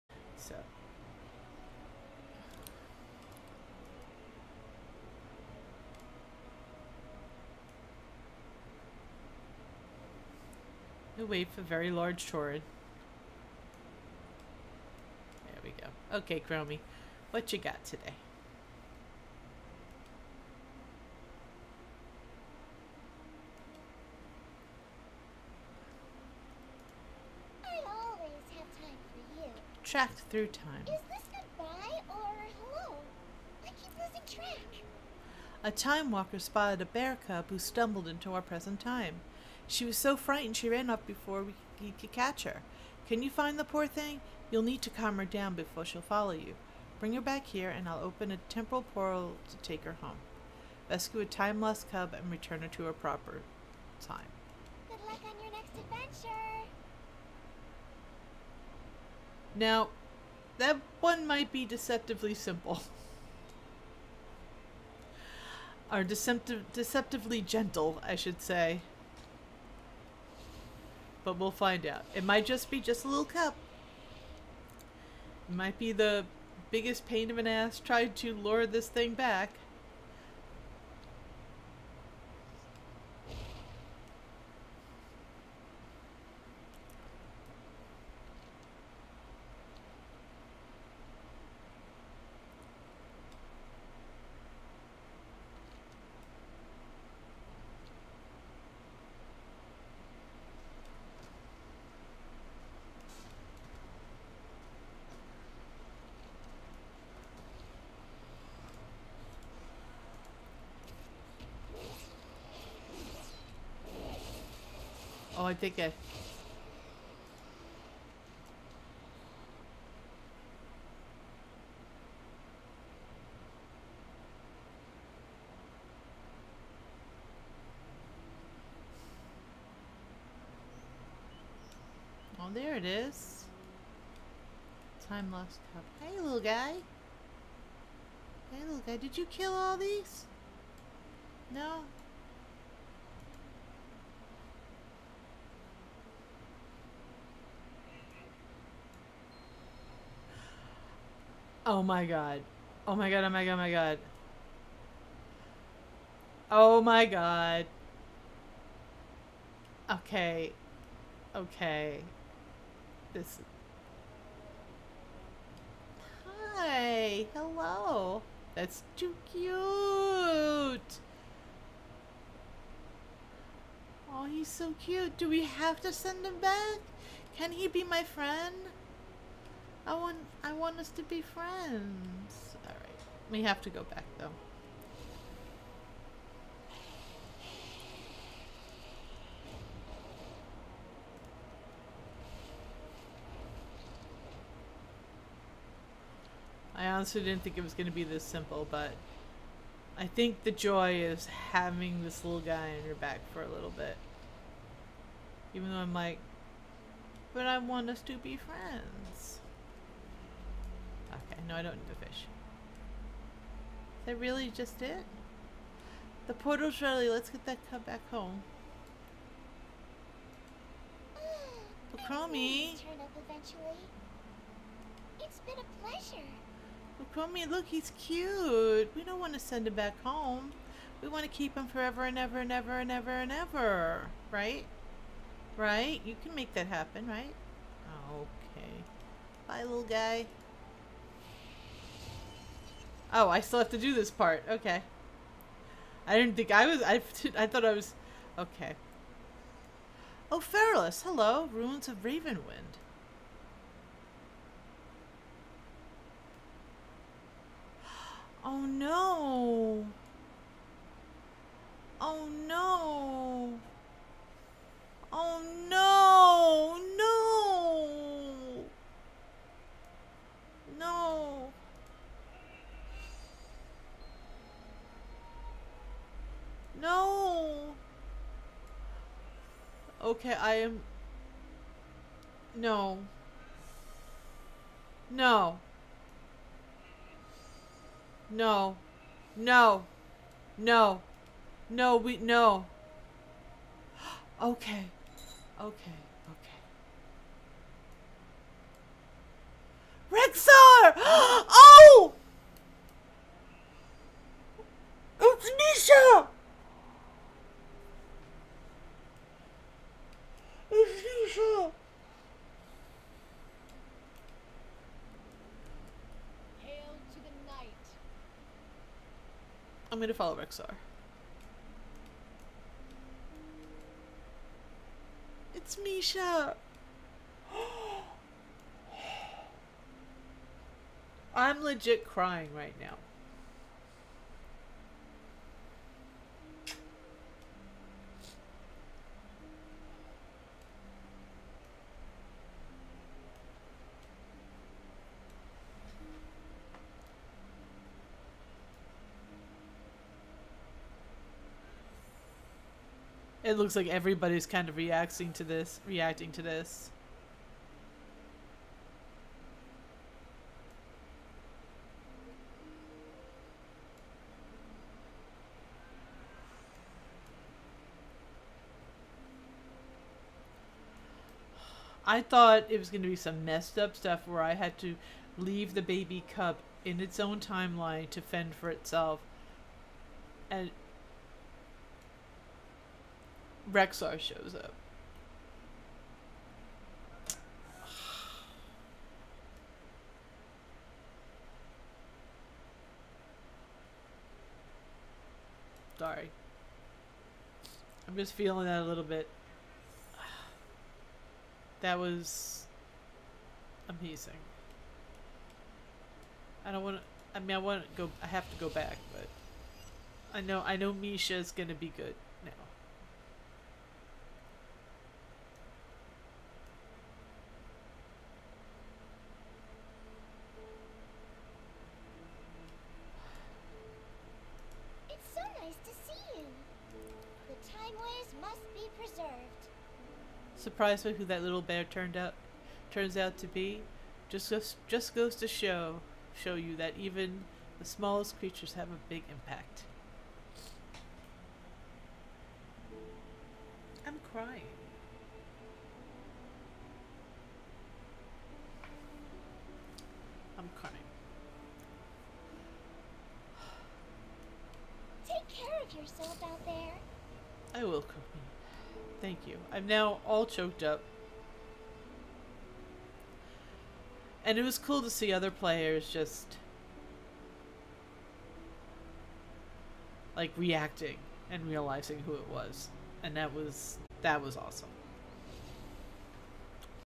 Highlight: Eon's Fringe: Tracked Through Time
This one actually made me cry. Rescue a bear cub and find her a new home (and BFF for life).